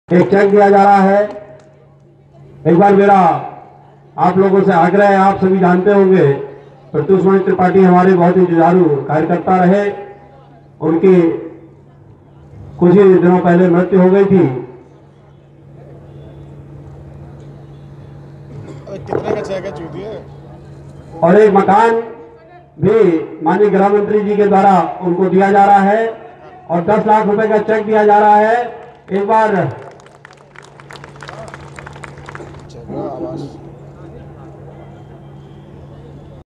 गृह मंत्री ने भाजयुमो नेता प्रत्यूष के परिवार को दिया आवास और दस लाख की सहायता
लखनऊ :  भाजयुमो के दिवंगत नेता प्रत्युषमणि त्रिपाठी के परिवार को आवास और दस लाख रुपए की सहायता गृह मंत्री राजनाथ सिंह ने दी. उल्लेखनीय है कि पिछले दिनों प्रत्युषमणि त्रिपाठी की हत्या हो गई थी.   जांच में पता चला था कि उन्होंने सुरक्षा पाने और अपने दुश्मनों को सजा दिलाने के उद्देश्य से अपने ऊपर हमला कराया था.